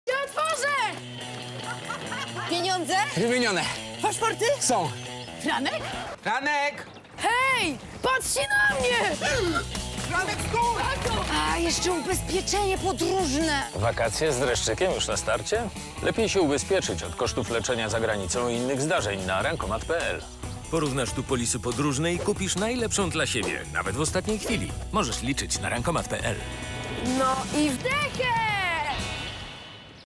Rankomat.pl - nie zapomnij o ubezpieczeniu podróżnym. Radzi Hubert Urbański
W najnowszej reklamie multiporównywarki rankomat.pl Hubert Urbański przypomina, że przed wyjazdem na wakacje warto się ubezpieczyć. Koszt polisy podróżnej nie musi być wysoki, a kupić można ją nawet tuż przed wyjazdem.